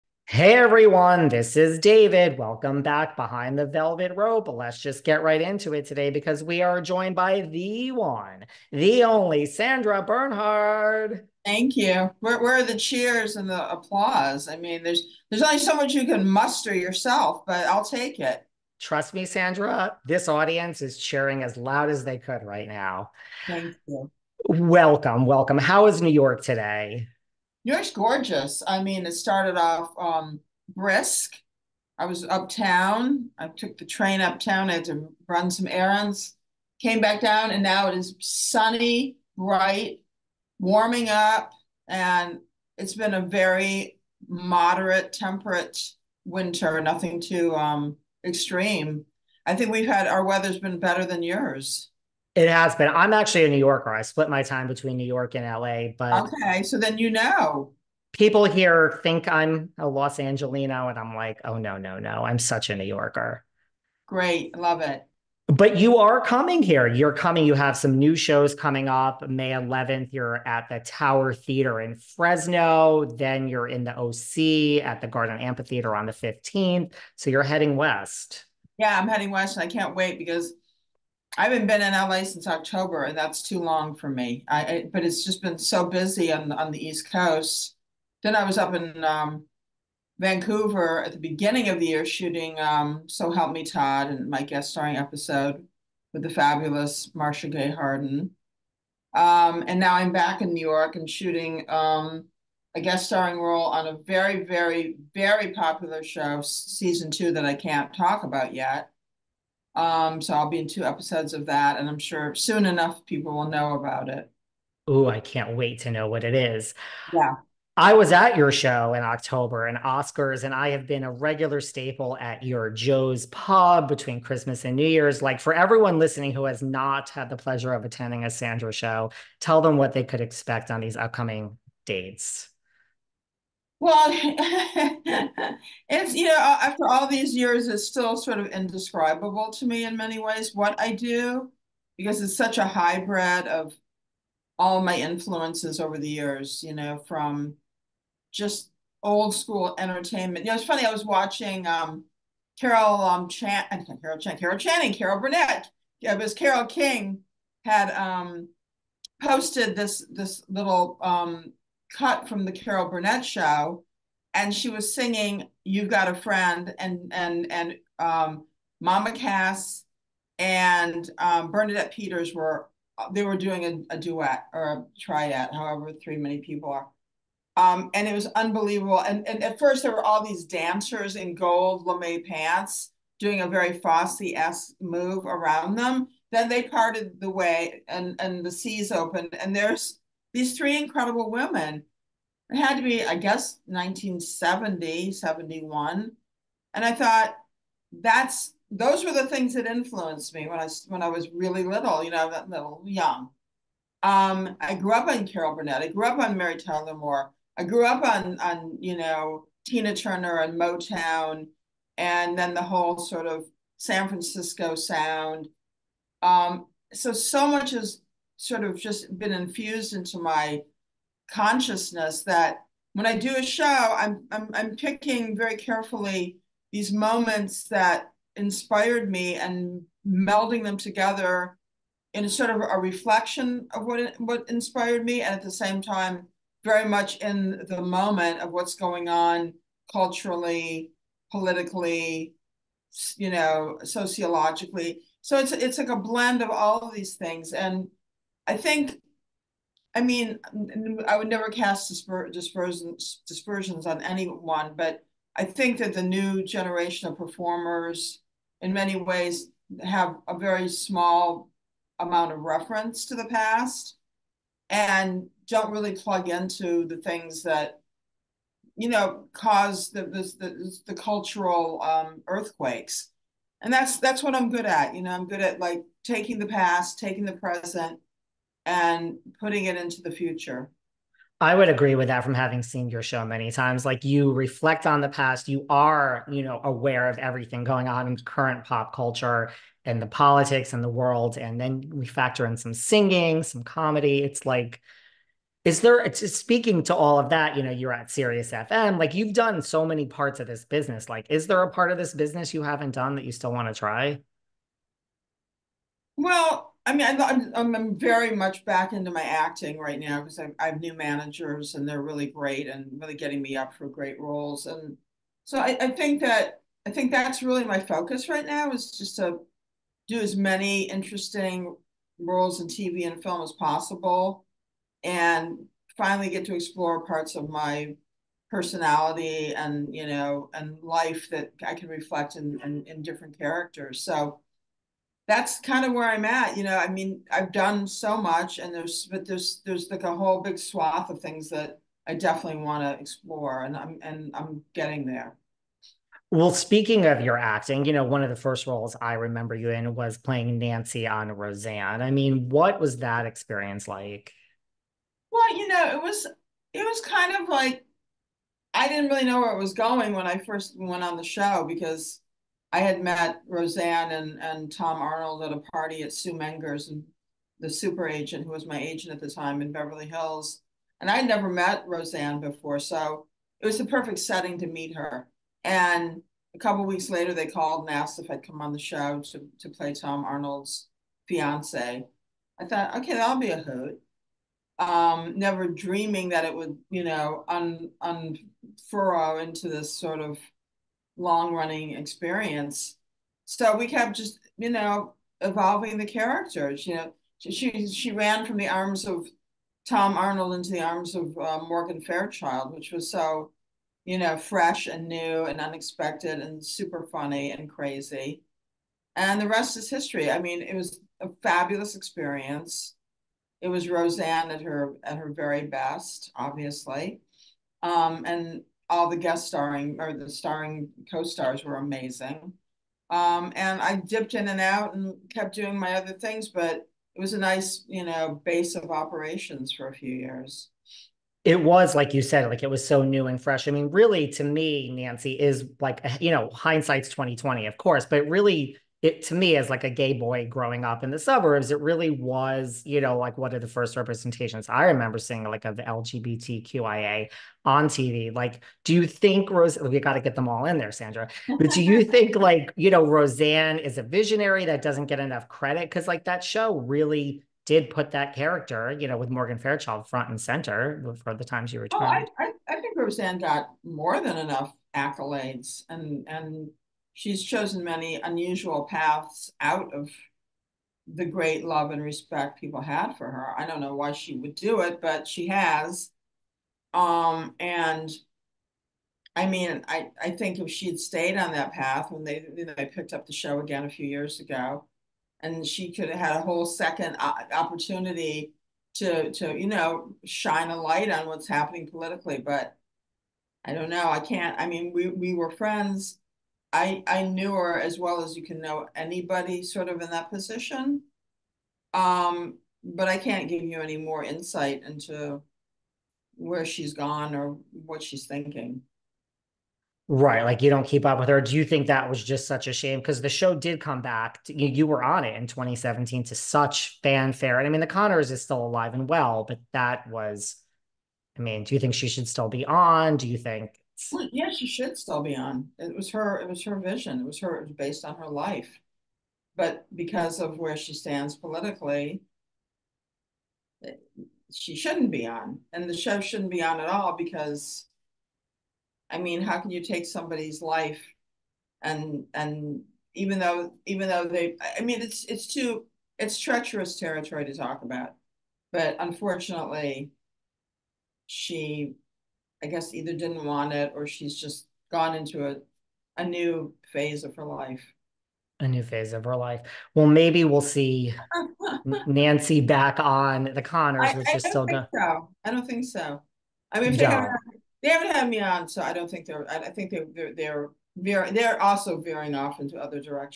Sandra Bernhard Shades Roseanne & Explains Why The Connors Should Be Off The Air
Sandra Bernhard steps Behind The Rope.  Actress, Comedian, Singer, Trailblazer… Icon! Sandra is here to mention it all. Sandra takes us through the full anthology of her career from the early days to break out moments such as “Without You I’m Nothing” to her current day live show - she is heading our way with a series of live shows this summer.  Sandra talks about her days on Roseanne, returning to the series in 2017, Roseanne’s cancellation and the current installation, “The Connors”. Sandra talks working with Ryan Murphy in stellar roles on both Pose and American Horror Story: NYC and LGBTQIA+ representation in the media today.  Of course Sandra talks her many regular appearances on Letterman, her much publicized friendship with Madonna, the aftermath of that friendship and where they stand today. Finally, Sandra talks Andy Cohen, Radio Andy, Sandyland and her many appearances on WWHL.